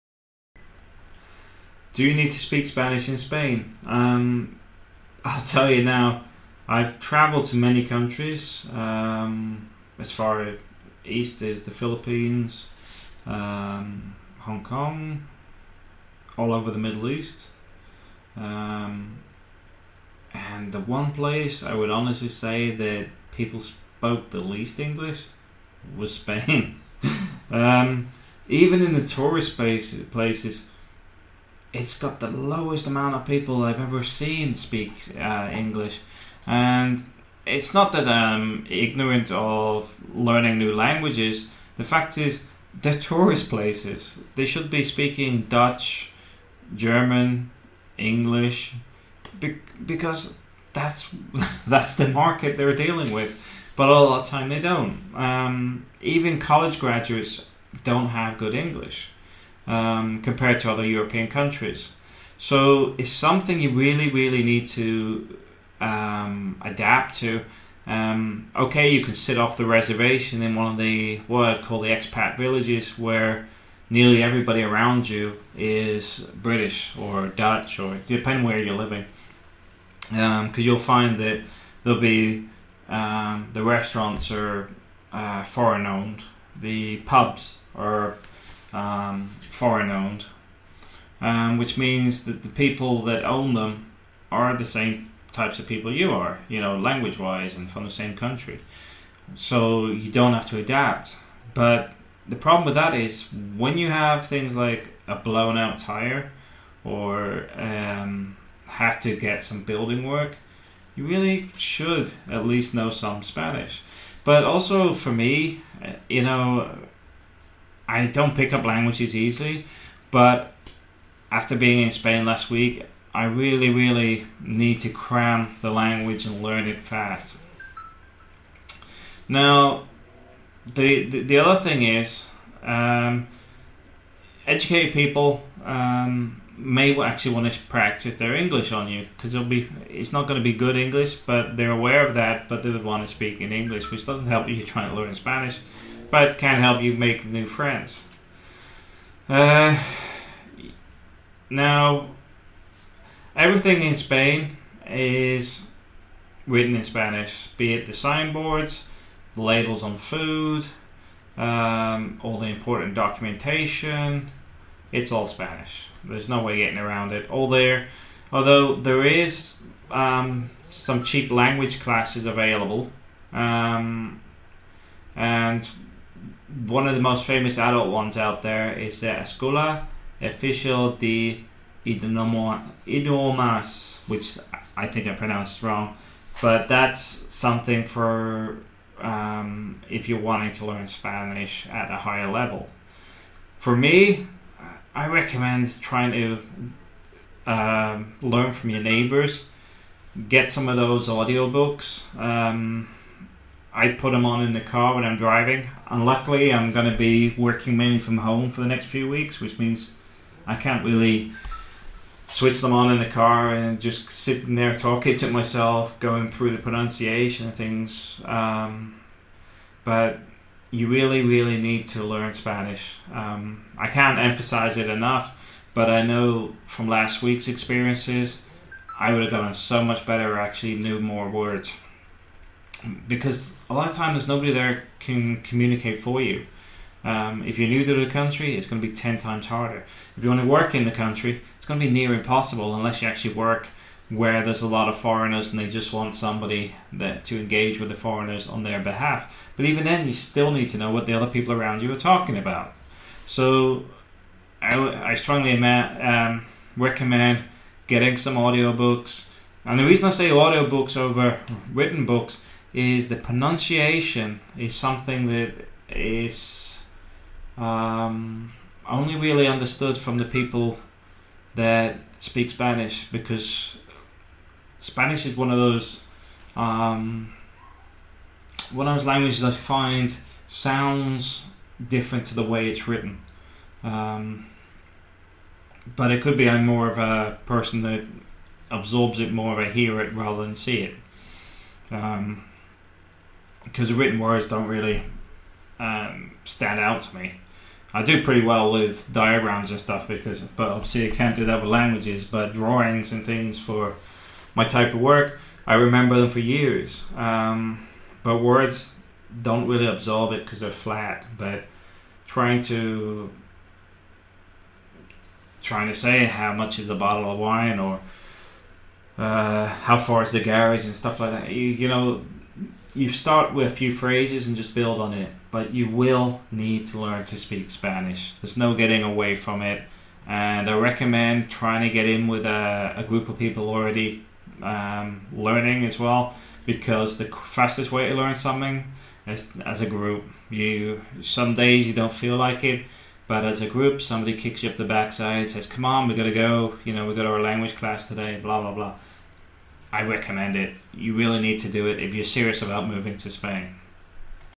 Top tips for moving to Spain - Do you need to speak Spanish in Spain
Do you need to speak Spanish if moving to Spain or on holiday? Fact is Spain is a country that I have heard the least amount of locals speak English than any other country I have visited.